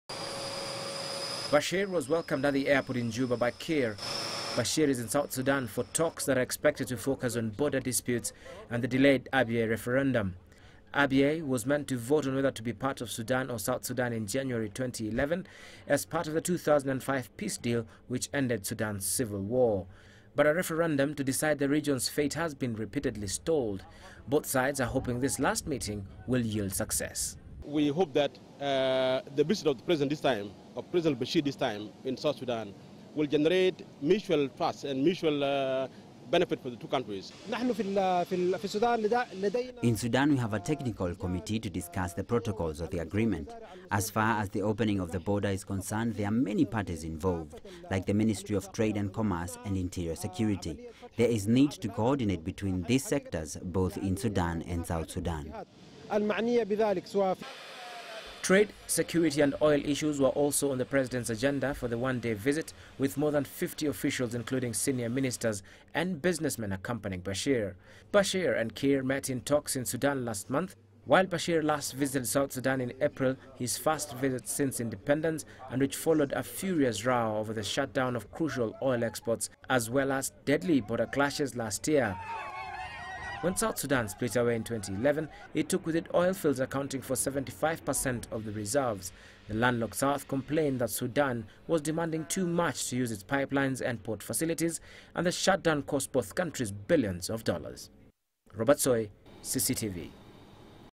Dialogue between presidents focused on trad,security and oil issues
A meeting between Sudanese President Omar Al Bashir and his South Sudanese  counterpart Salva Kiir in Juba has failed to resolve the differences between their two countries on the fate of the disputed Abyei region. The leaders however reiterated their commitment to fully implement a cooperation agreement brokered by the African Union.  In this regard borders between the two  countries will be reopened to allow free movement of people and goods. Ministers incharge of interior affairs in both countries are required to ensure this agreement is fully implemented by mid November. Robert Soi now reports.